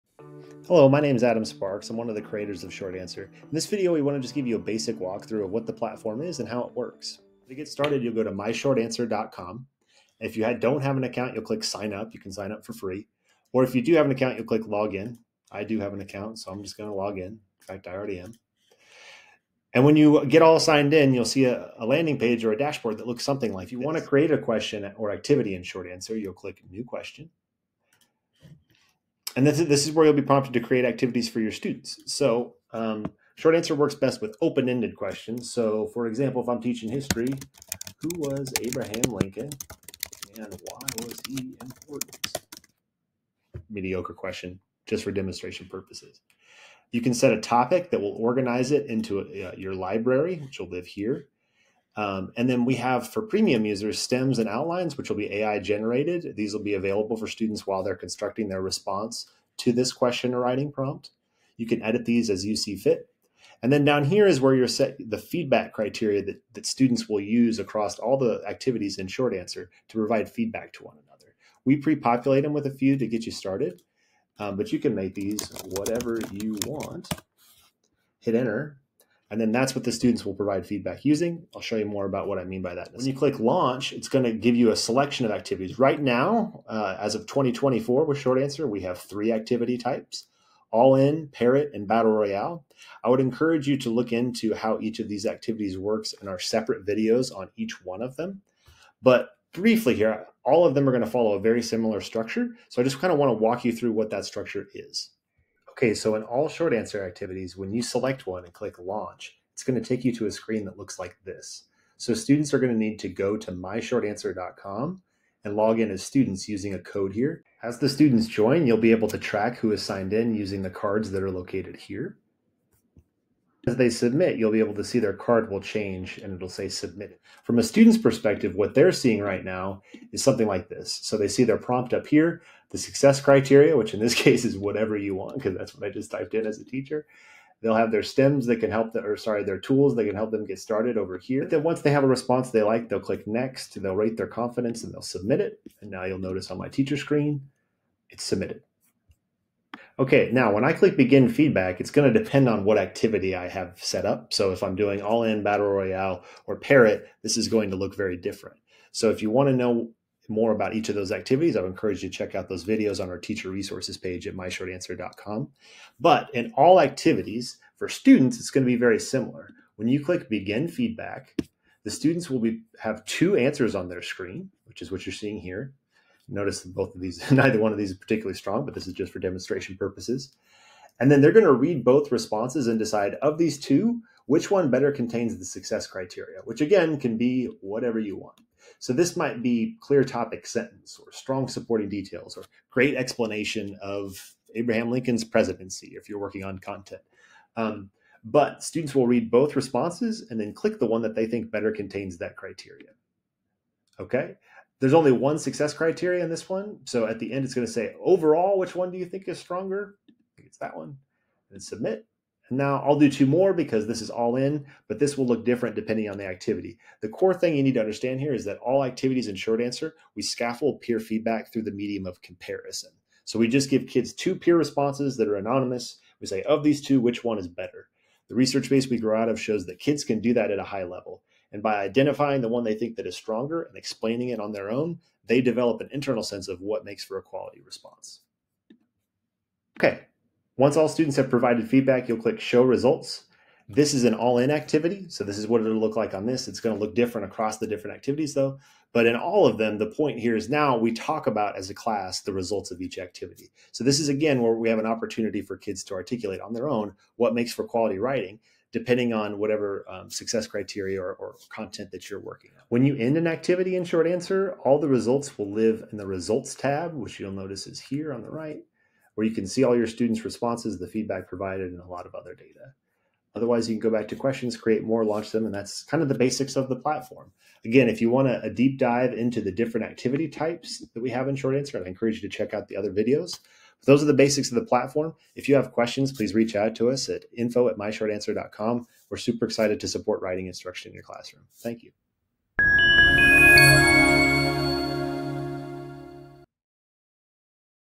Getting started with Short Answer
In this 5-minute overview, Short Answer co-founder Adam Sparks walks you through how to set up, launch, and facilitate an activity in Short Answer.